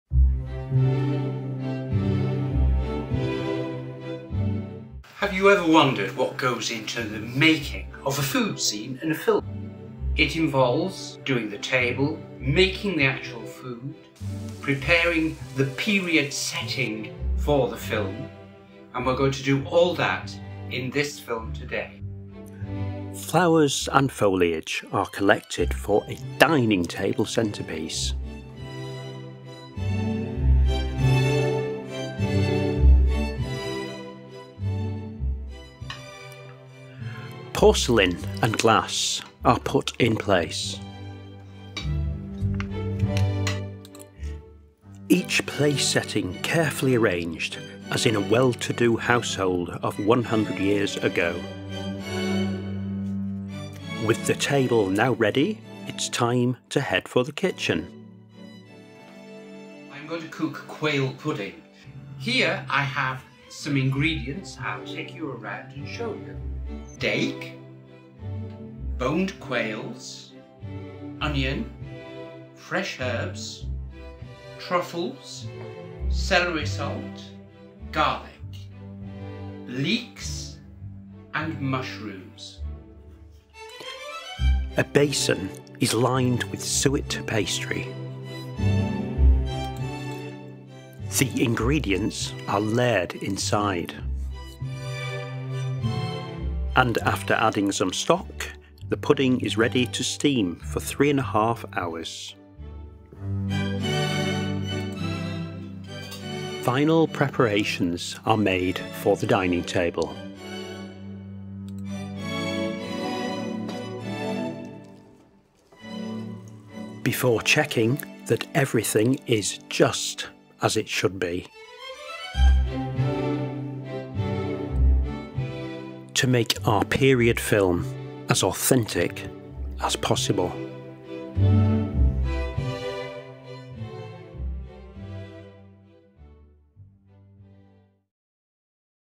Dressing Film Set for a Dinner Party
Film set preparation for a period dinner party in anticipation of Res et Artes short film  later this year